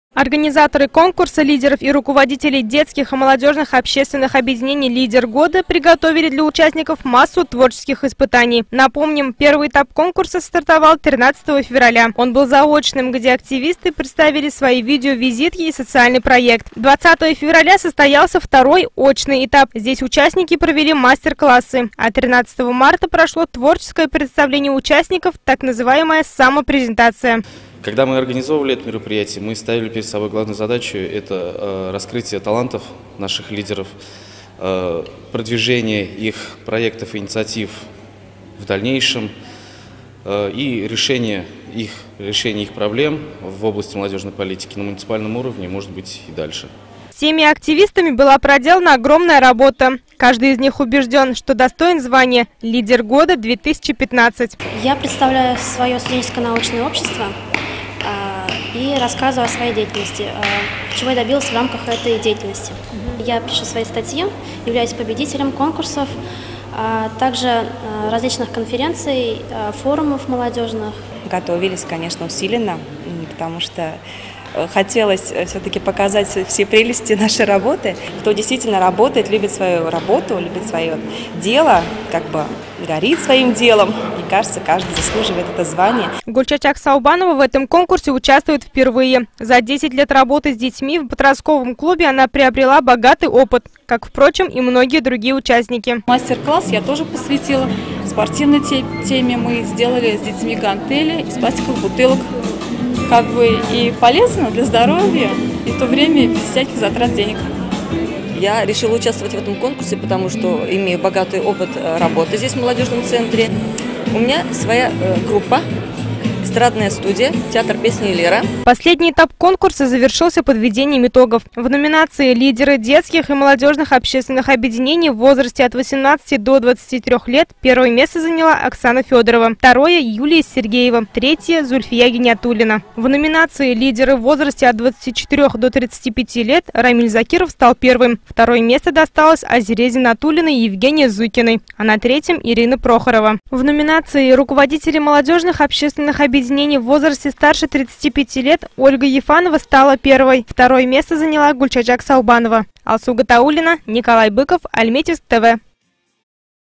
В Альметьевске назвали лидеров года
Талантливые и успешные руководители, студенты-активисты, представители молодежных комитетов предприятий показали свое мастерство на конкурсе «Лидер года-2015» в Альметьевске. За это звание боролись более 50 человек.